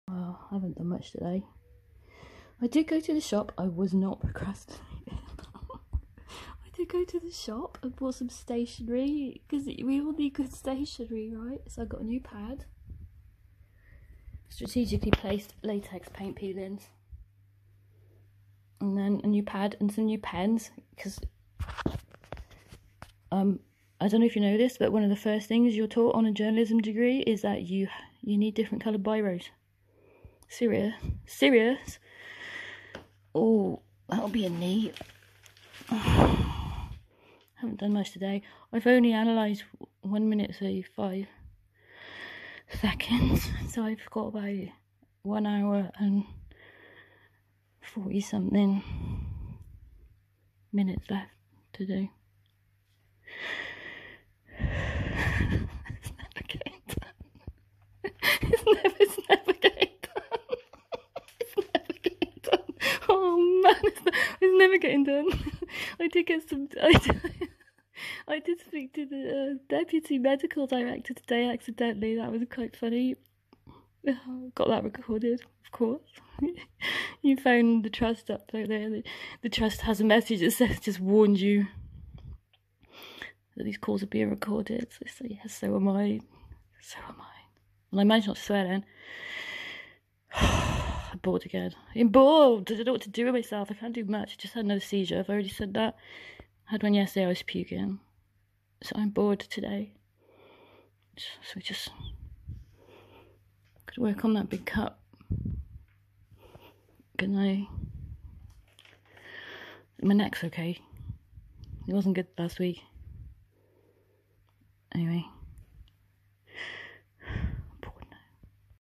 55 seconds, actually, but...How's the NHSExposé build for YouSeeUs going? 🙄
I AM going to try working again shortly.  Just a quick waffle while taking a break after another (some sort of seizure).

But, I REALLY should not be laughing.
It HAS to get done.